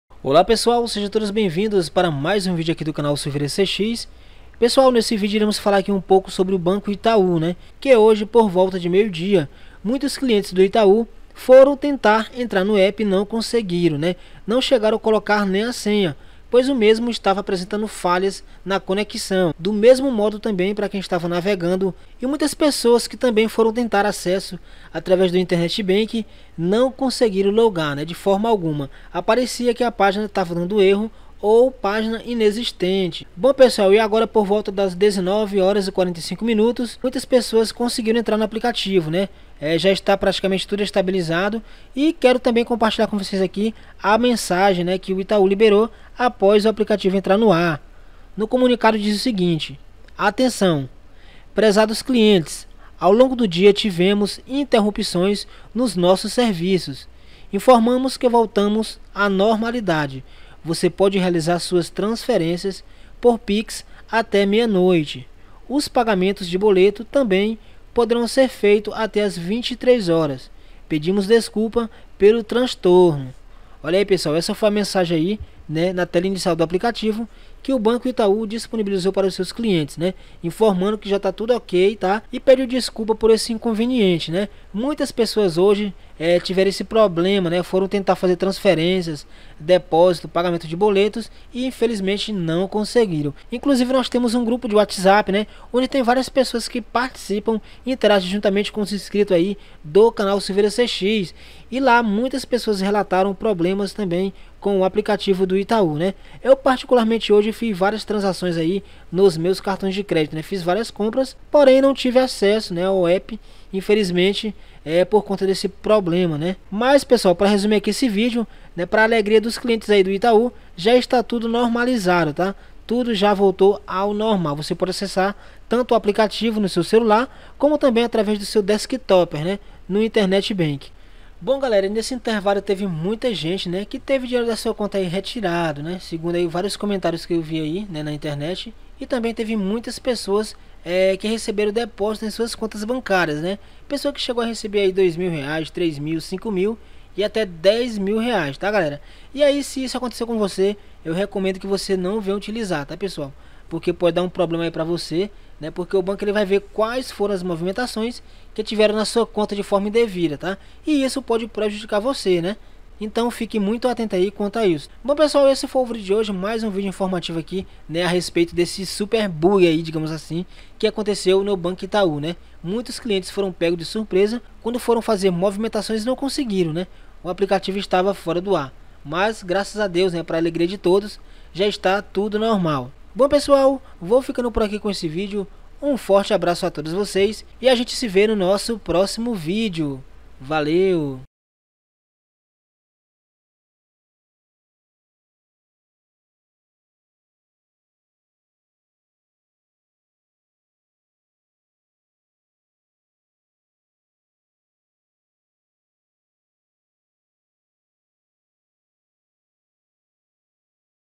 URGENTE! BANCO ITAÚ FUNÇÕES FINALMENTE VOLTOU A FUNCIONAR
NESSE DIA 03/03/2022 O BANCO ITAÚ  FICOU COM SUAS FUNCÕES INDISPONÍVEIS TEMPORÁRIAMENTE E HOJE POR VOLTA DAS 19:45 VOLTOU A FUNCIONAR NORMALMENTE. VEJA MENSAGEM QUE O BANCO ENVIU NESSE VIDEO...